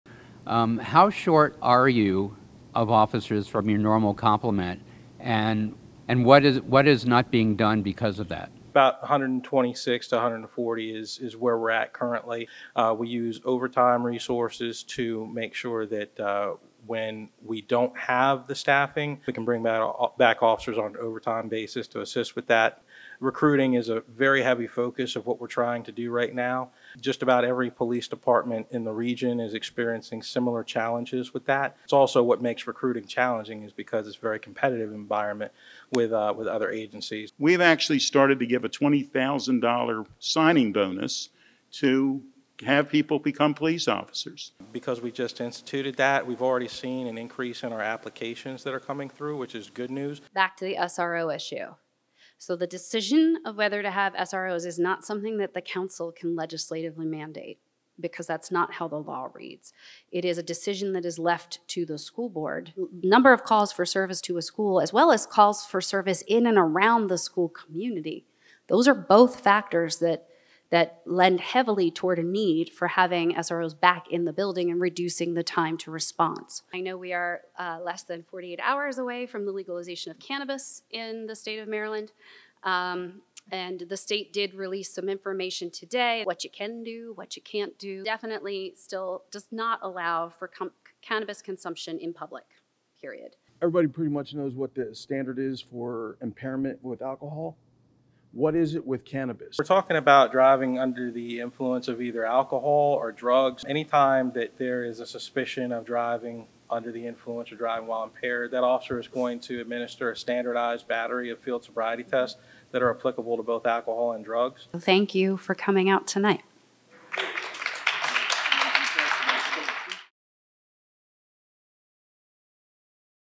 Luedtke Town Hall on Public Safety
Montgomery County Councilmember Dawn Luedtke held a public safety town hall to address citizens concerns.